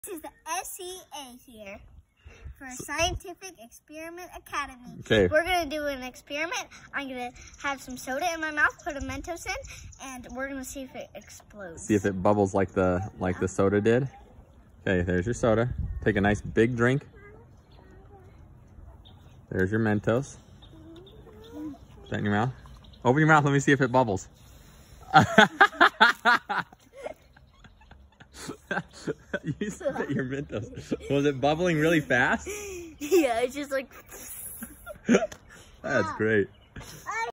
Kids Mentos and Soda Experiment
Everybody knows what happens when you put a mentos in soda, but what about if that soda is in your mouth? Sounds like an experiment for the S.E.A.